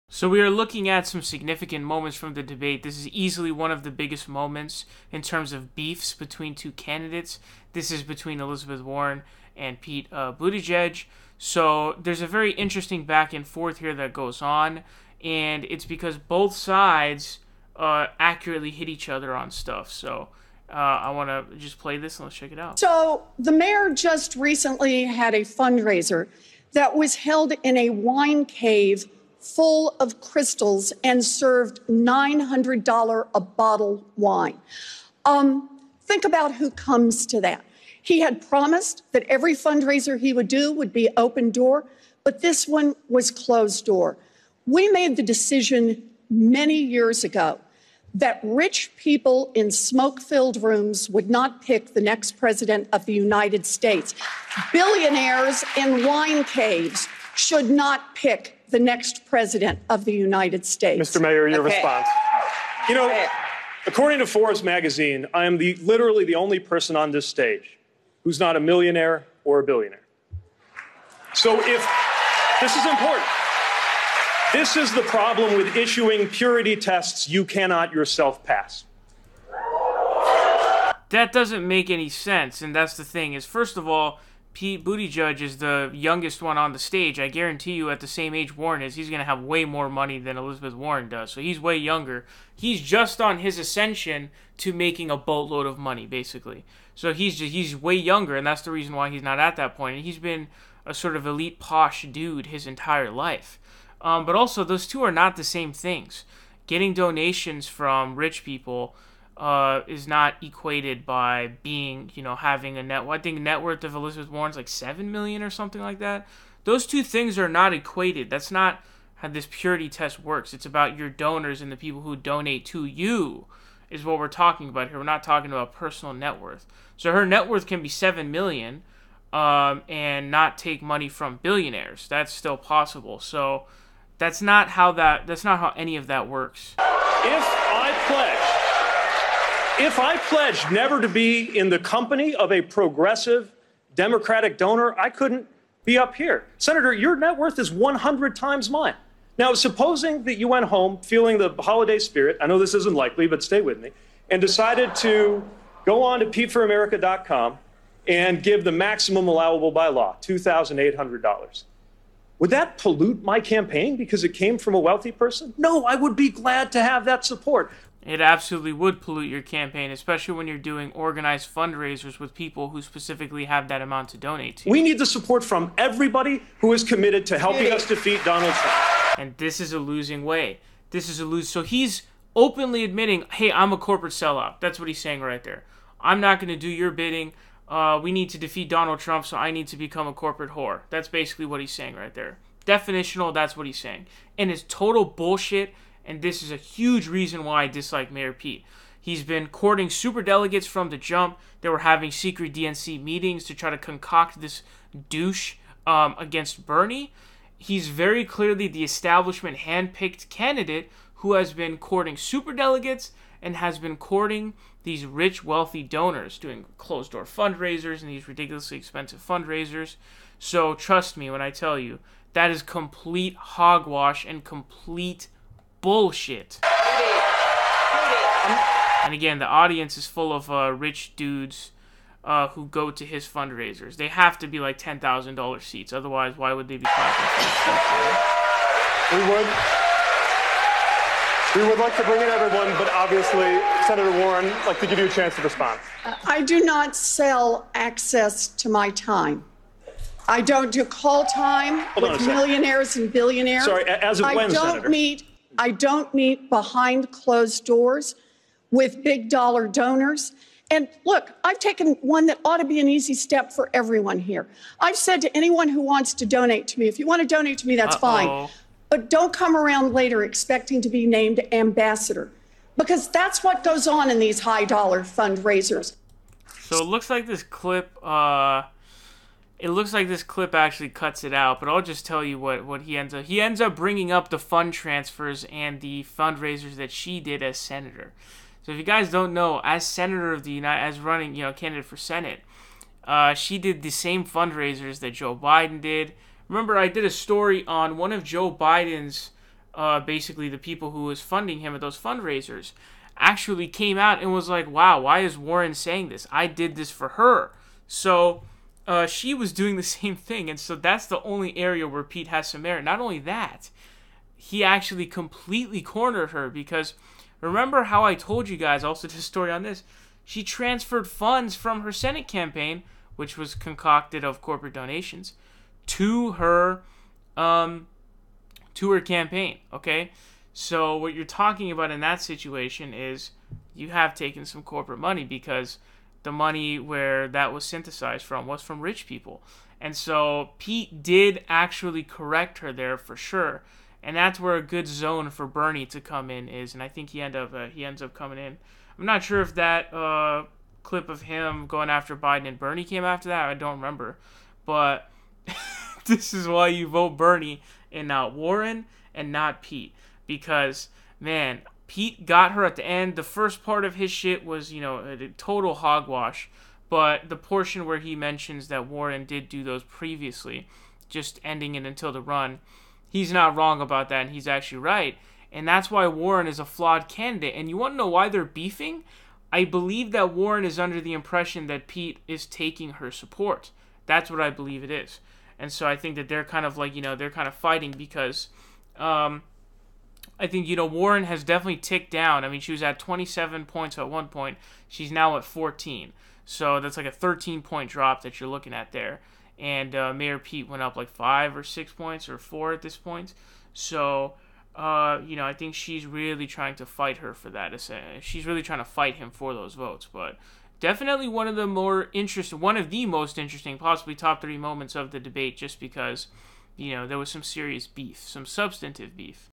Elizabeth Warren & Pete Buttigieg CLASH & They Both EXPOSE Each Other For Being Sellouts!
Elizabeth Warren turned Pete Buttigieg’s wining and dining of donors, namely his recent fundraiser inside a swanky Napa Valley wine cave, into a punchy argument against his presidential campaign during Thursday’s Democratic debate.

“Billionaires in wine caves,” Warren said in the midst of a heated exchange at Loyola Marymount University in Los Angeles, “should not pick the next president of the United States.” Photos of the fundraiser, popping with crystal chandeliers, went viral this week, drawing virtual smirks online and a quick rebuke via a small-dollar fundraising plea from Bernie Sanders. With Buttigieg and Warren competing for similar high-education voters in Iowa, and with Buttigieg overtaking Warren in polls in the first caucus state, the Massachusetts senator saw an opportunity to burnish her populist credentials at the expense of the mayor of South Bend, Indiana.

“So the mayor just recently held a fundraiser that was held in a wine cave full of crystals and [that] served $900-a-bottle wine,” Warren began — after obliquely referencing Buttigieg’s fundraising practices a moment earlier. “Think about who comes to that. He had promised that every fundraiser he would do would be open-door, but this one was closed-door.”

The clash had been brewing for weeks. Warren, who does not hold high-dollar fundraisers, pushed for Buttigieg to open his donor events to reporters — a level of transparency that former vice president Joe Biden had been adhering to since the spring. Buttigieg’s campaign relented soon after, and a reporter actually attended a portion of the wine cave event, though the fundraiser was otherwise closed to those who hadn’t donated at least $500.

“Those doors shouldn’t be closed, and no one should be left to wonder what kind of promises are being made to the people that then pony up big bucks to be in the room,” Warren said earlier this month.

Buttigieg has parried Warren’s criticism by casting her as a hypocrite, given her personal wealth and her acceptance of high-dollar fundraisers before her presidential campaign. On the debate stage Thursday night, he also offered what amounted to an extended defense of pricey fundraisers, asserting that Democrats need all the help they can get if they want to beat President Donald Trump.

“This is the problem with issuing purity tests you cannot yourself pass,” Buttigieg said to Warren, who stood to his immediate left. “If I pledge never to be in the company of a progressive Democratic donor, I couldn’t be up here.”Buttigieg then turned toward Warren with a personal challenge: “Senator,” he said, “your net worth is 100 times mine. Now supposing that you went home feeling the holiday spirit — and I know this isn’t likely, but stay with me — and decided to go on to PeteForAmerica.com and give the maximum allowable by law, $2,800, would that pollute my campaign because it came from a wealthy person? No, I would be glad to have that support.”

Warren shot back: “I do not sell access to my time.”

Buttigieg, who got the last word in the nearly five-minute exchange, then noted that Warren had transferred money from her Senate campaign to her presidential campaign — money she raised during a time in which she participated in “big-ticket fundraisers.” At the start of her 2020 bid, she transferred $10.4 million from her 2018 Senate campaign to her presidential account

Ethereum: 0x7699AE5E4A4b491Cf2cF3dAc3EE14Adf8c29b438